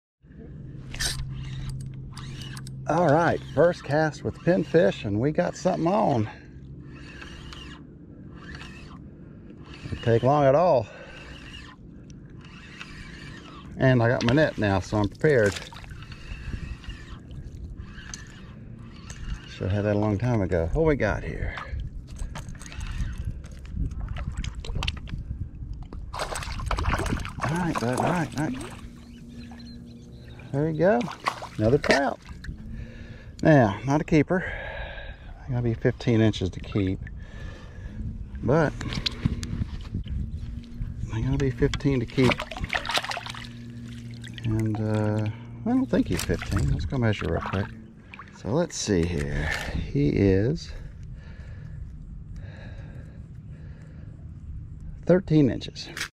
Speckled trout on popping cork
I caught some pinfish in my cast net and used them with a popping cork - great setup for speckled trout on St. Andrews Bay - Panama City, FL. floridafishing saltwaterfishing speckledtrout @andrewdixon8612